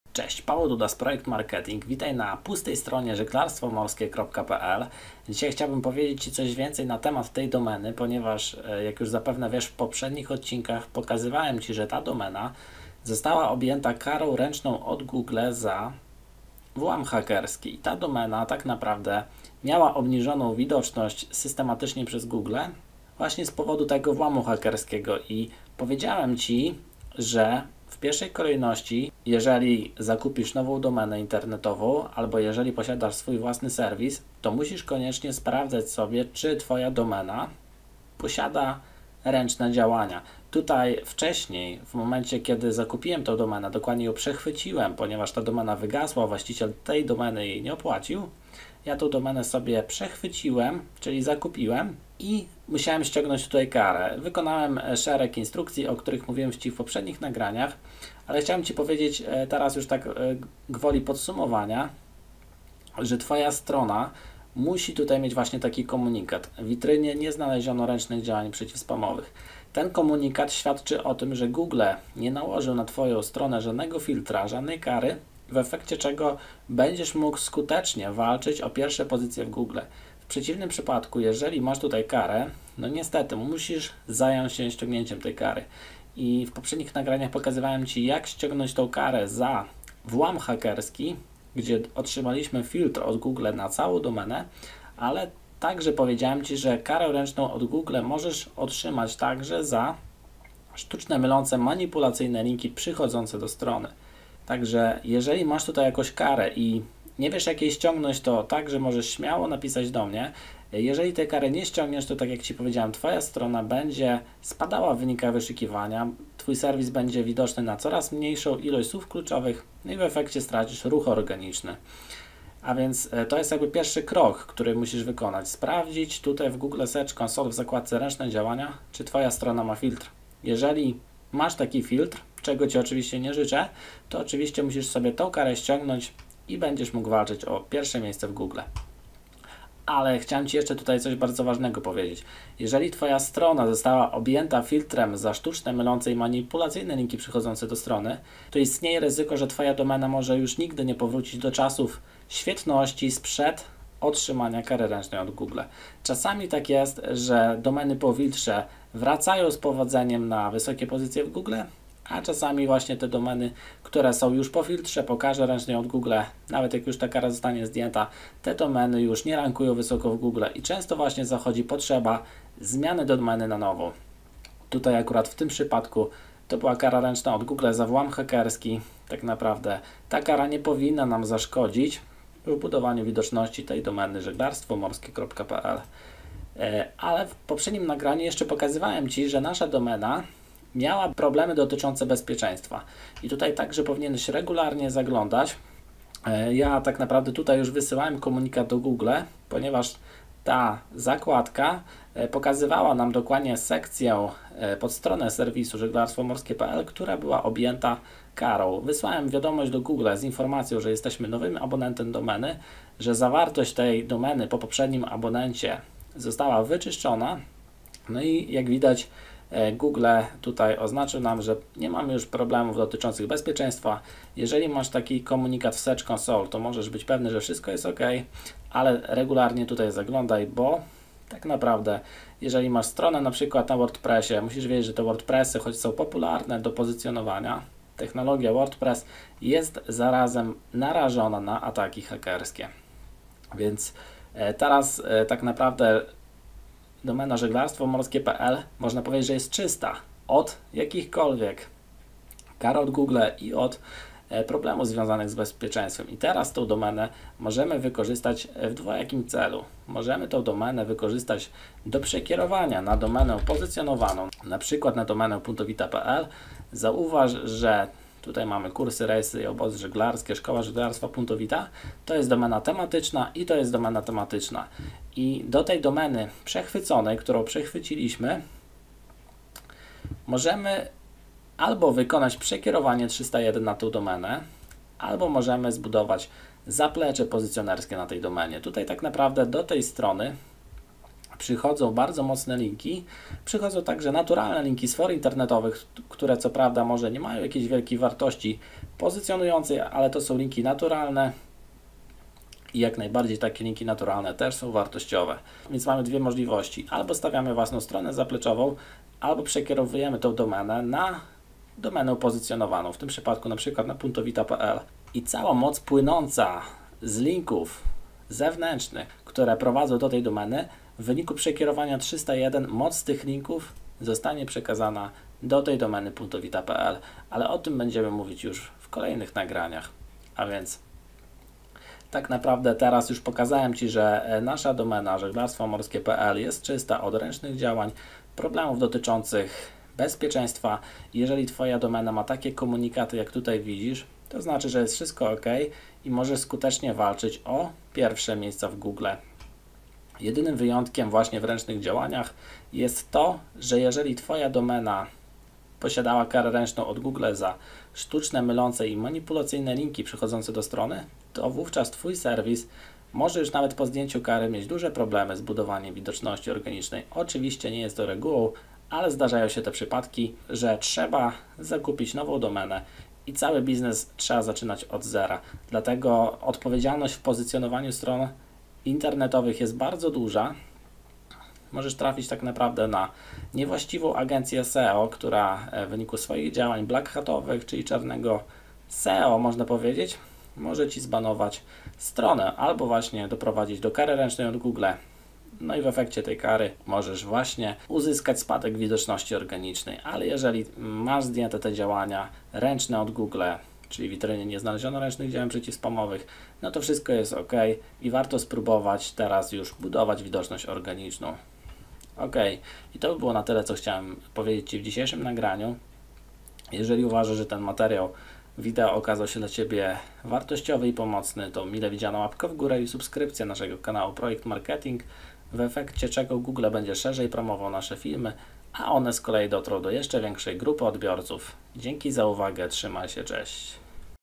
Dlaczego warto ściągnąć karę ręczną od Google i naprawić luki w zabezpieczeniach witryny?
Zobacz wideo poradnik, dlaczego warto zdjąć filtr od Google? Jakie są konsekwencje, gdy zlekceważysz karę od Google?